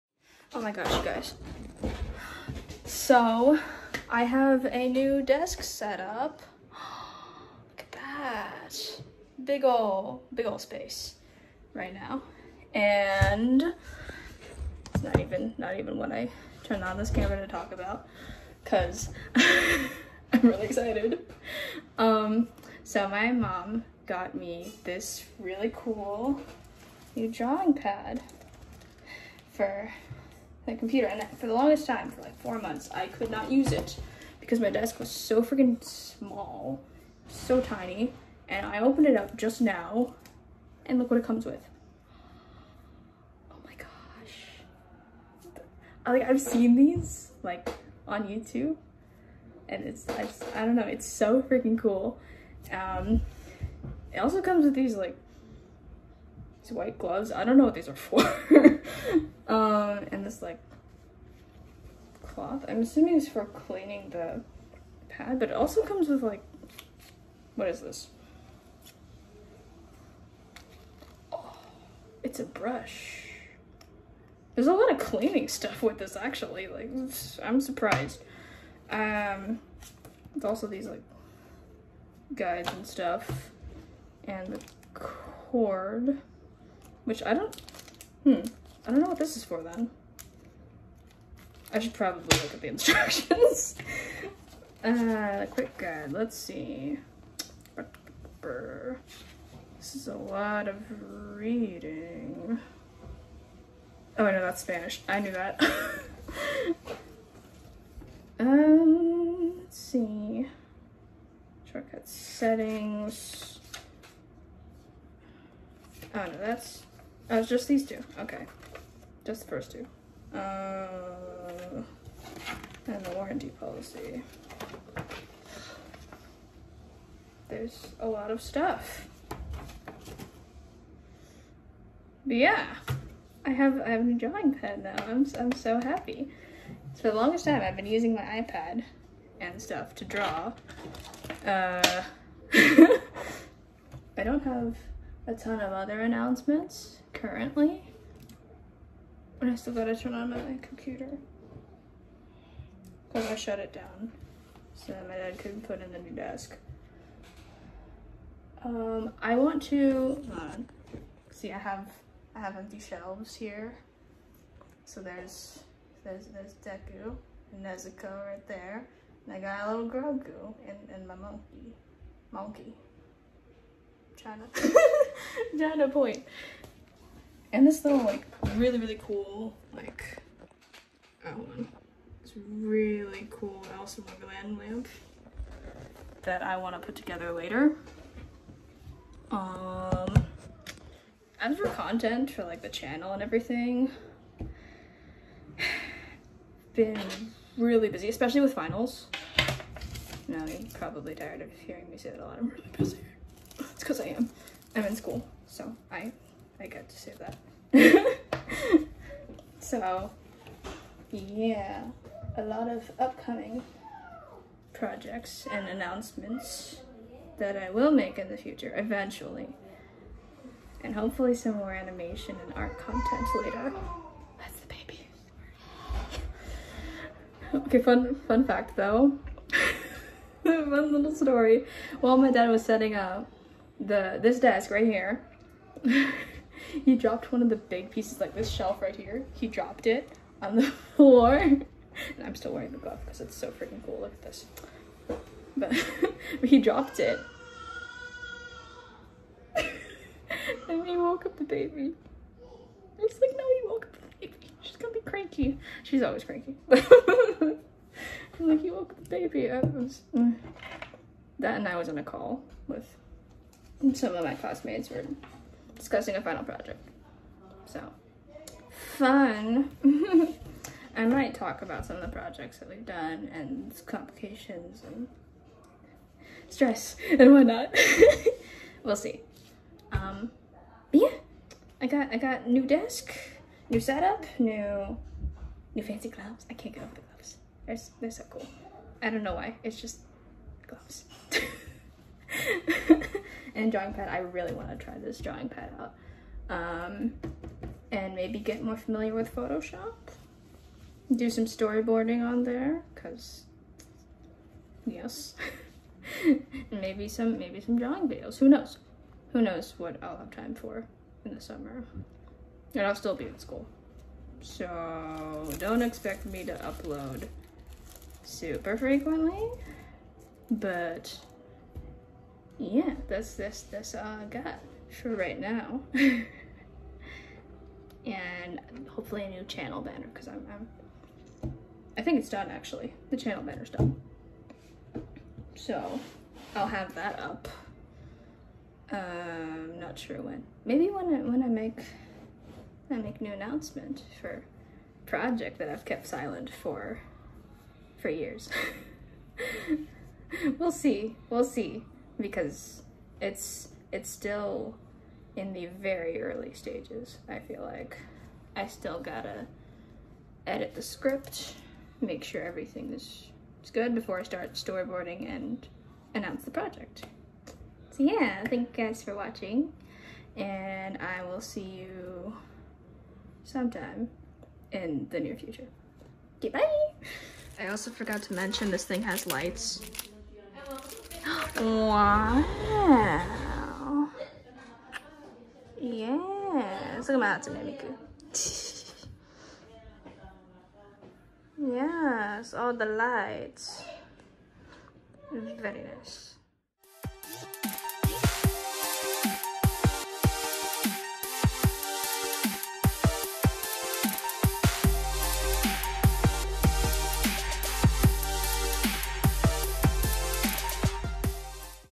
New Desk and Drawing pad Mini Vlog
Hey guys! My upload schedule as you know is kinda nonexistent at the moment. So enjoy this mini vlog showcasing my new set up and drawing pad! Also we moved to a new house a while ago, which is why my room looks different so yeah!